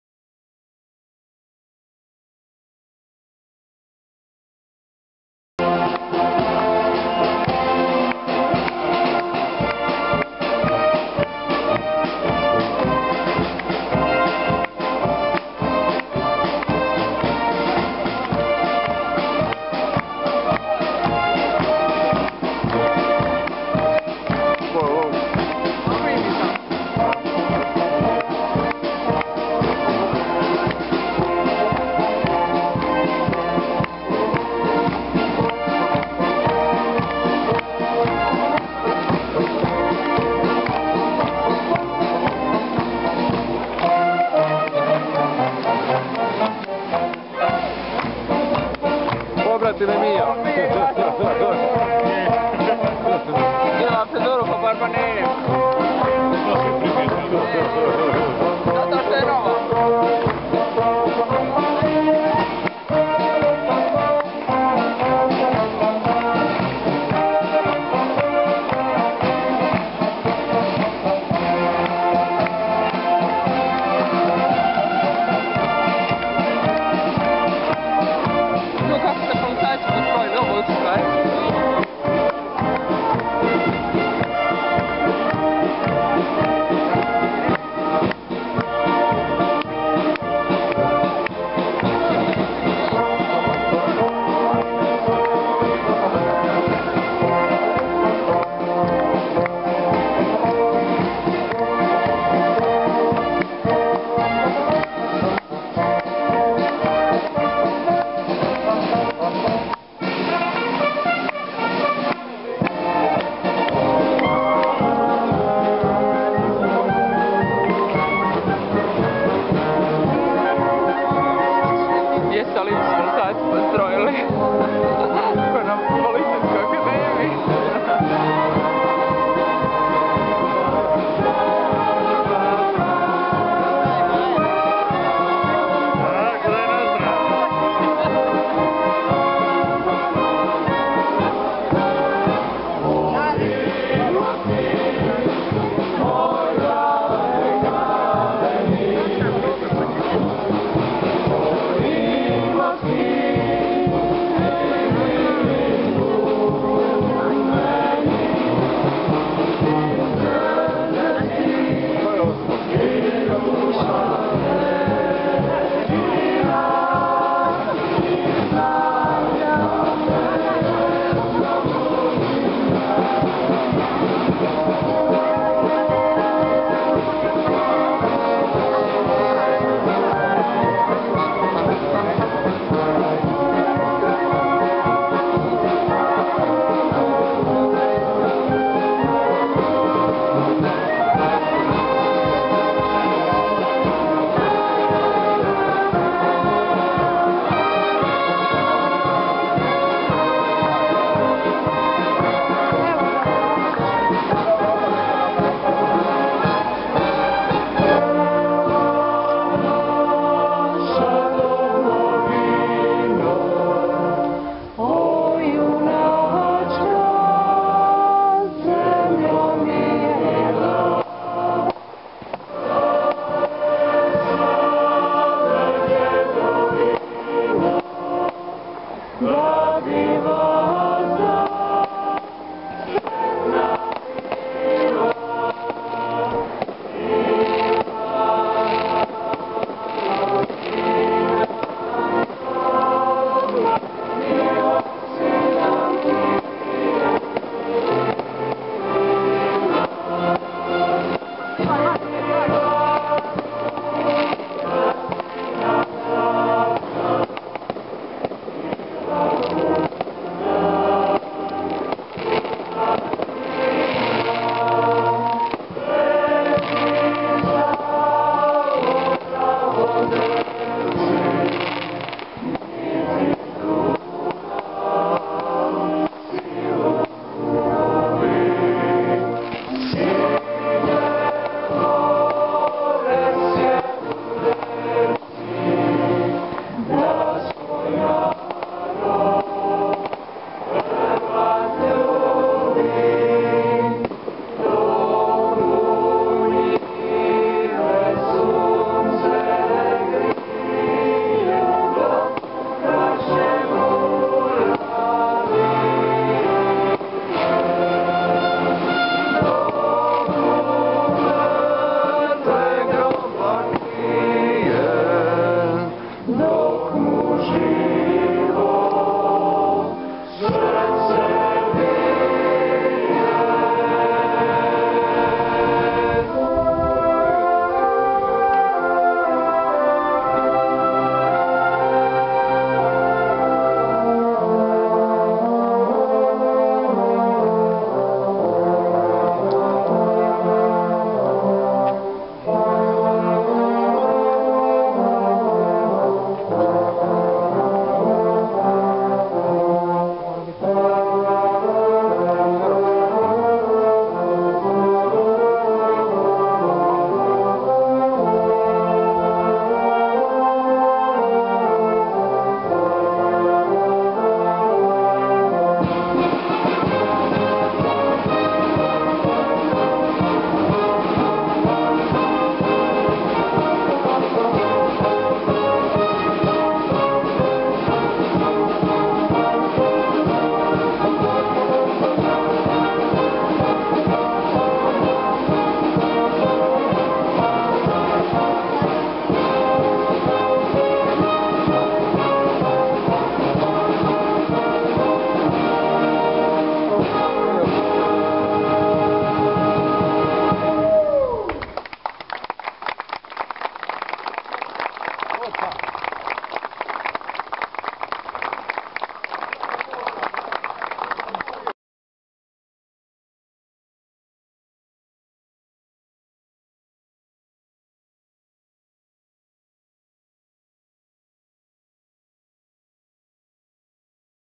Ulazak Hrvatske u Europsku Uniju - Granični prijelaz Vinjani Donji
Zastavu Europske unije podignuo je gradonačelnik grada Imotskog Ante Đuzel uz pratnju Hrvatskog puhačkog orkestra - Gradska Glazba Imotski.